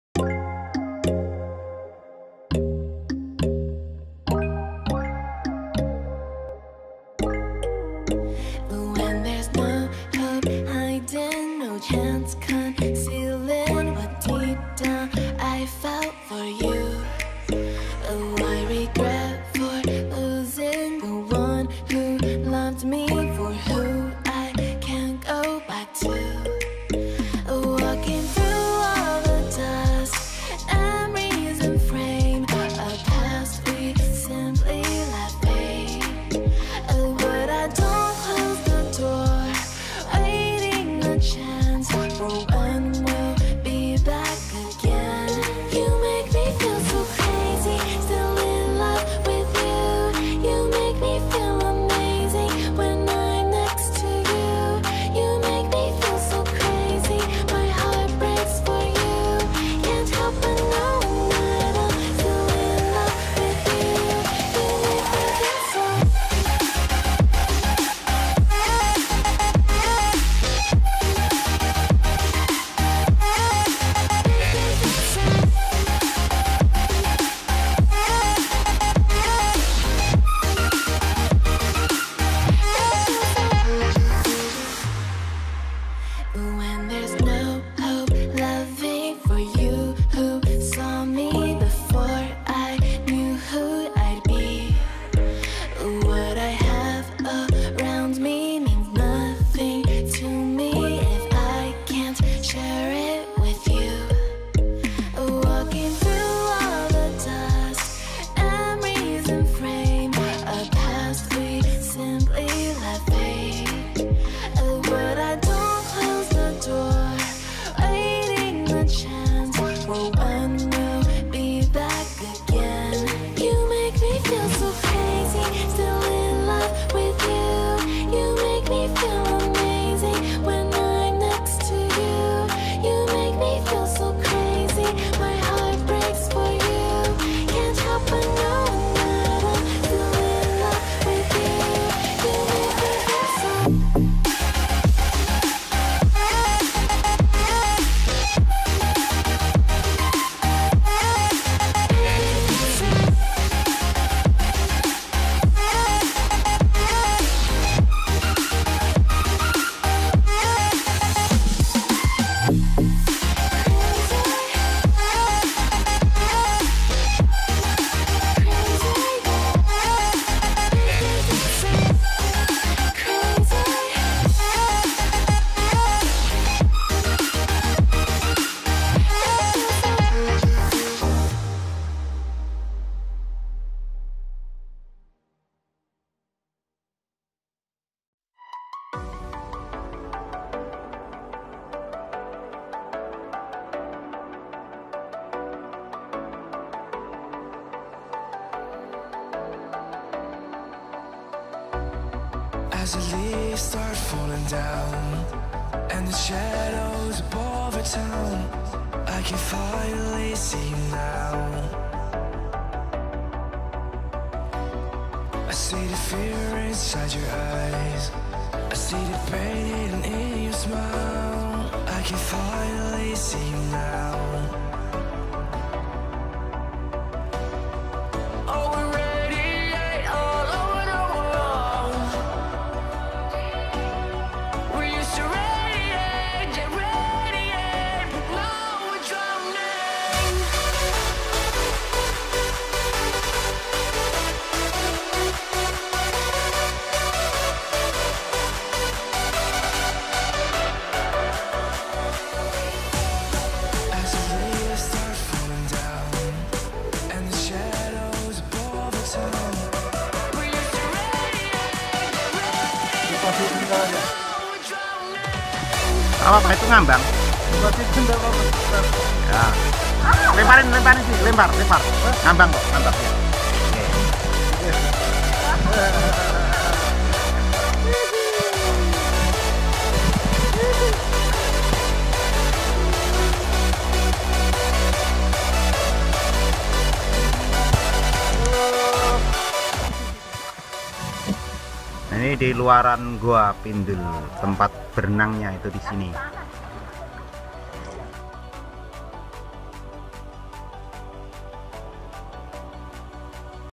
Keseruan di Goa Pindul Gunung Kidul Yogyakarta - Tempat Wisata Alam Jogja
Goa Pindul terletak di Jogja , tepatnya desa Gelaran 1, Kelurahan Bejiharjo, Kabupaten Gunung Kidul.